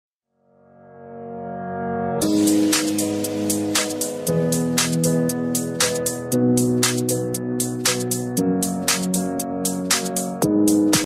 Super Bright LED Headlights V15 Tested in Car
90Watt 10000Lumen
CSP3570 LED Chips
Fast Cooling System
H1/H3/H4/H7/H11/9005/9006/9012